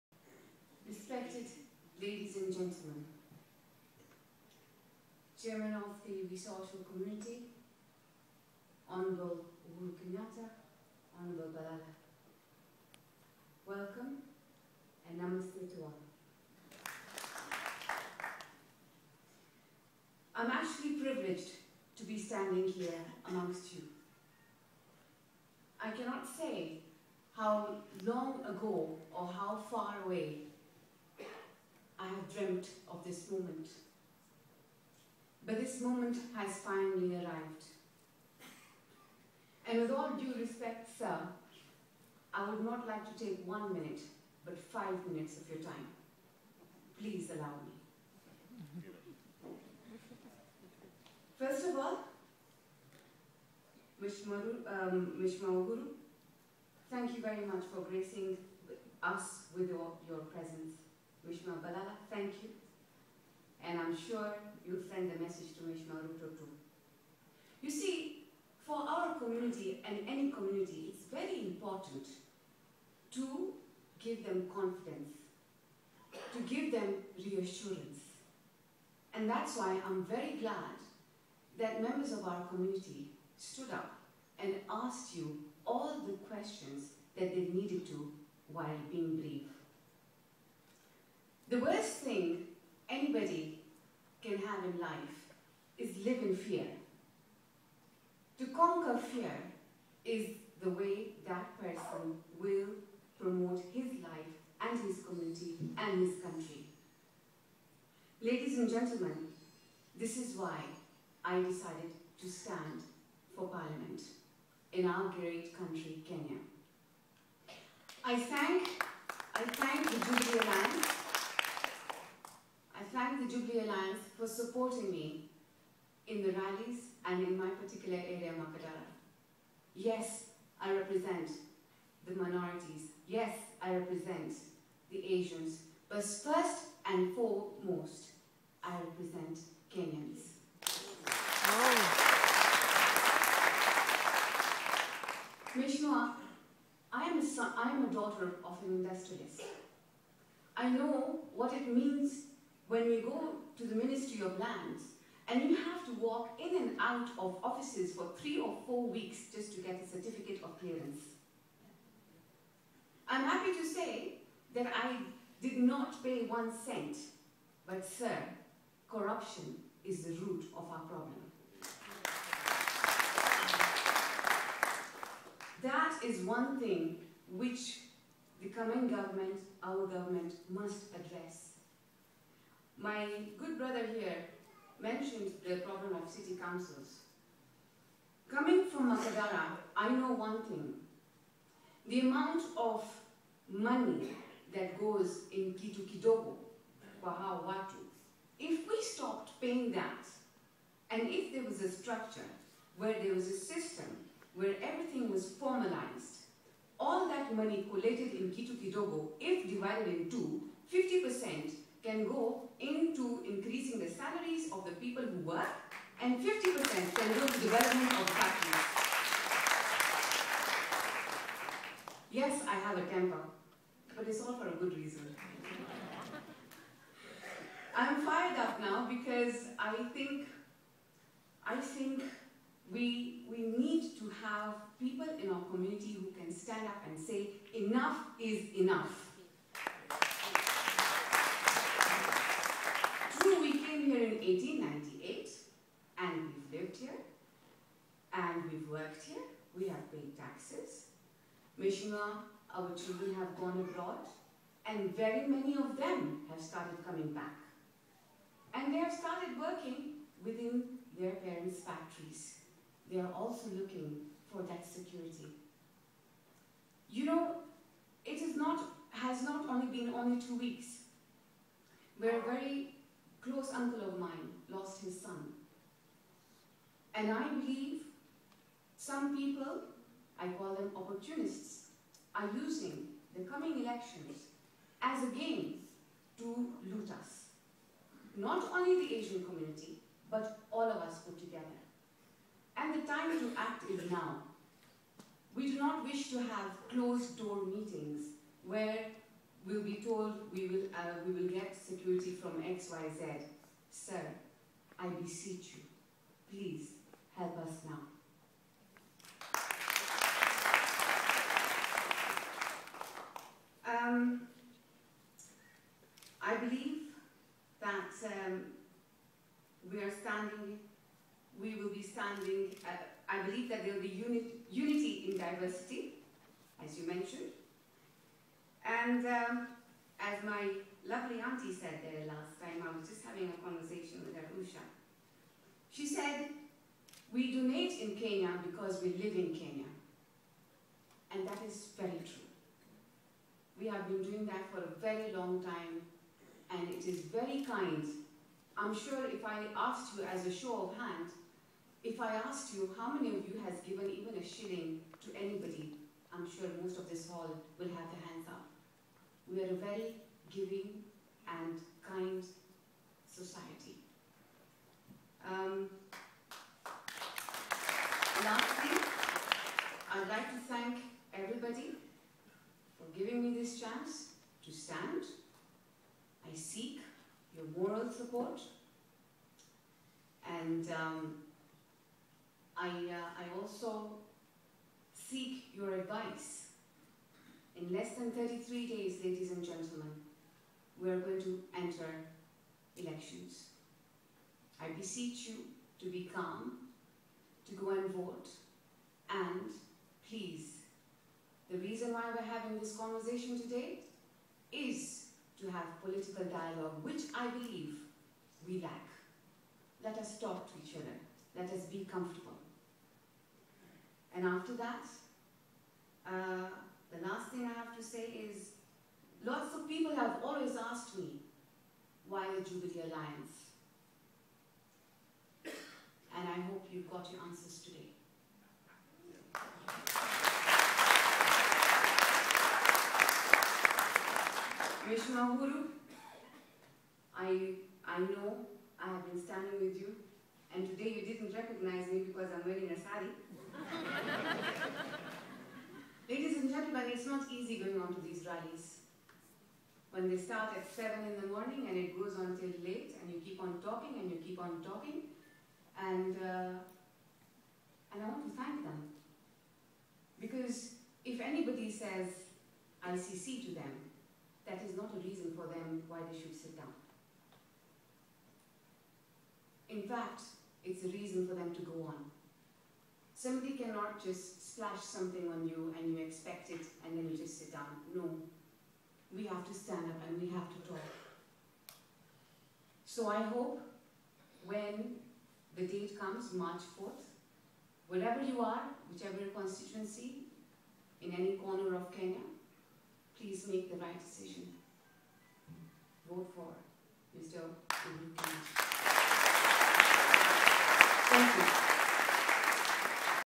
Sonia Birdi Addresses Hindu Council of Kenya, Jubilee Coalition and Members of the Asian Community
Sonia Birdi is the URP candidate for Makadara MP Seat in the Upcoming Kenyan General Elections 2013. She talks about Asians facing constant insecurity problems and City Council nagging.